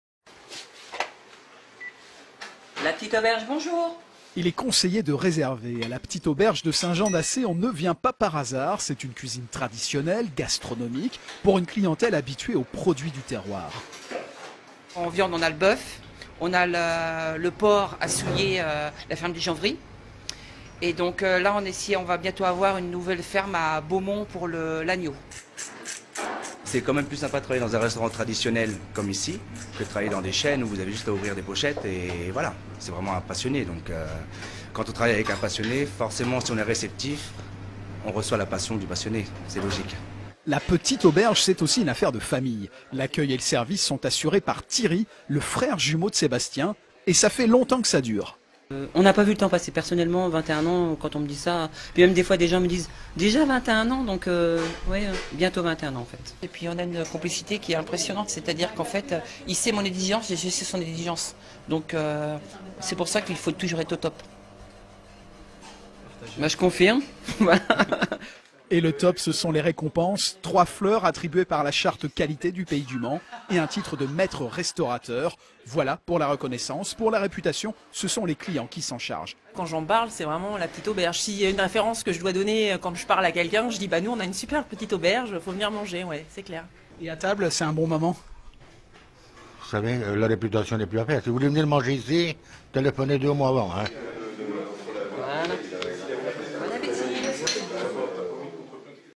La petite Auberge de Saint Jean d'Assé
Un titre de Maître Restaurateur et une troisième fleur attribuée par la charte Qualité et proximité du Pays du Mans;
La Petite Auberge de Saint-Jean D'Assé collectionne les récompenses et attire des clients de tout le département depuis 21 ans.